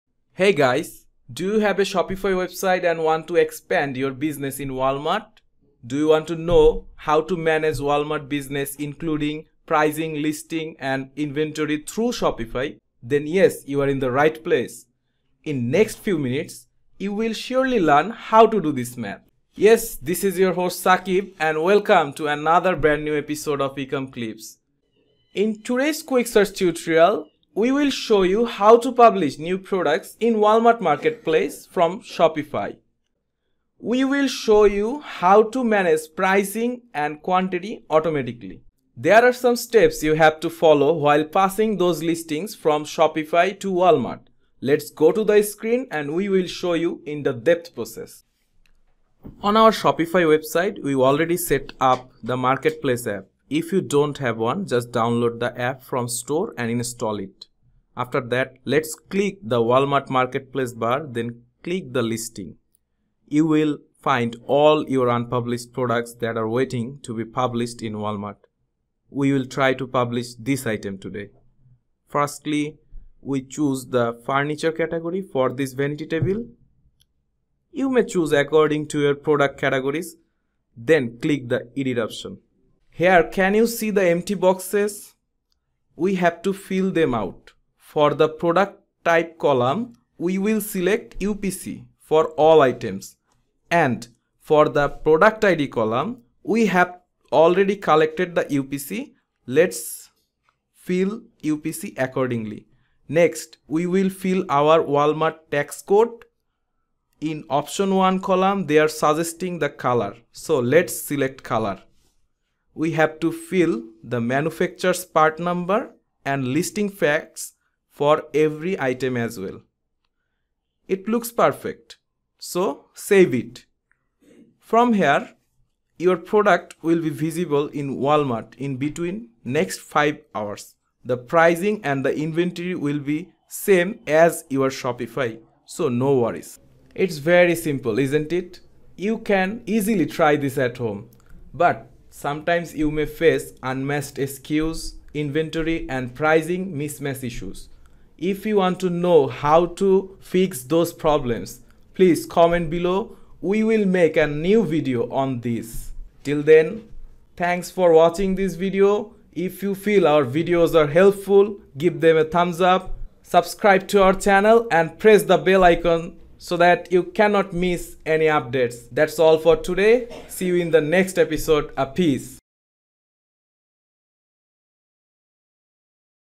List Products on Walmart Via Shopify - Add Listings on Walmart Marketplace Using Shopify 2023
Shopify is one of the biggest platforms to develop an online shop brand. Shopify app store has so many integration apps. These apps help to manage your other e-commerce marketplaces like Walmart, eBay, Etsy, and others. So we can publish products in Walmart from Shopify. Shopify has a Walmart marketplace app for product listing passing, Walmart inventory managing, and pricing set up inside your Walmart seller central according to your Shopify prices automatically.

While passing Shopify listings to Walmart, we must follow some steps, like selecting the product from the unpublished item list, select the categories. Then have to go to the edit options. Here for every specific item, we have to give every item a specific product ID type from UPC, EAN, GTIN, or ISBN. We prefer UPC here. Also, we have to fill in the other attributes. For example, you made the color variation for some Shopify products. So you have to pass those variations with accuracy, that’s why you have to fill out all the black boxes to publish products in Walmart from Shopify. Once every attribute is filled in. you can save those all together.

Now your integration is over from Shopify to Walmart. After saving the product, your Walmart will catch all the information you saved previously in this integration. Walmart will also catch all your Shopify product text descriptions, pricing, inventory. But no worries, you can find the useful option in the Walmart marketplace app in Shopify. If you don’t want to pass your pricing automatically or inventory, you can turn them off. For more detail please follow the below steps...

On our Shopify website, we already set up the Walmart marketplace app.

Step 1: After that, let’s click the Walmart marketplace bar then click listings. You will find all your unpublished products that are waiting to be published in Walmart. We will try to publish this item today. Firstly we choose the furniture category for this vanity desk. You may choose according to your product categories. Then click the edit options.

Step 2: Here can you see empty boxes? We have to fill them out. And For the product type column, we will select UPC for all items, and for the product id column, we have already collected the UPCs ( notepad open). Let’s fill the UPC accordingly. Next, we will fill in our tax code, in option one column, they are suggesting the color, So let’s select color. We have to fill in the manufacturer’s part number and lighting facts for every item as well.

It looks perfect. So save it. From here, your product will be visible in your Walmart seller central in-between the next 5 hours. The pricing and the inventory will be the same as your Shopify.

That’s all. We think this video will help you to manage your business using Walmart App in Shopify. If you have any more queries don’t hesitate to ask us by commenting here in the comment section.

Timestamps

00:00 - Intro & Topic
00:48 - Work Procedures
02:21 - About our next video